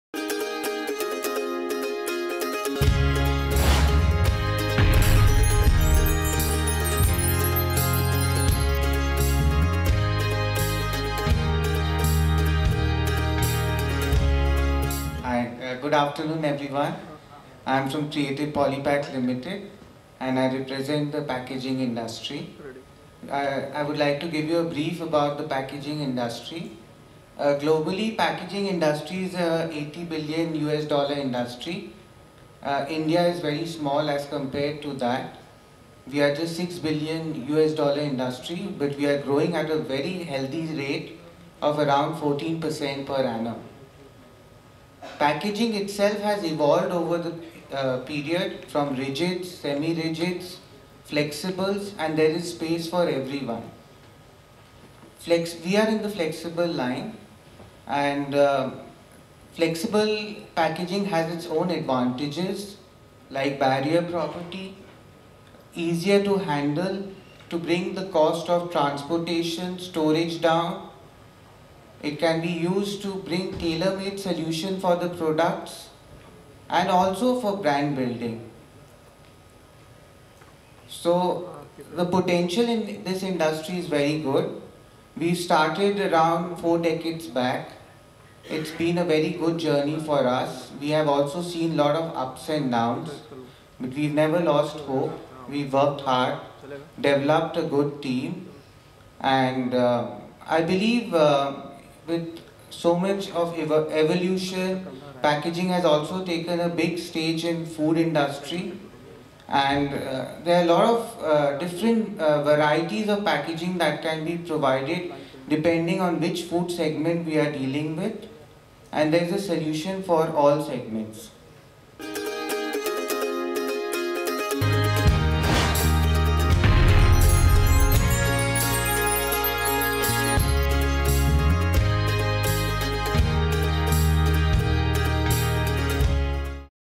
Mr. Giriraj Maheswari, Executive Director, Creative Polypack Limited
Creative Polypack Limited is India's leading flexible packaging company with 8 state-of-the-art manufacturing facilities in strategically planned locations across the country and production capacity of over 26,400 MT per annum. Since its inception in 1986 at Kolkata, Creative Polypack caters to evolving flexible packaging needs of FMCG companies, both in India and overseas and has a turnover of over 400 crores.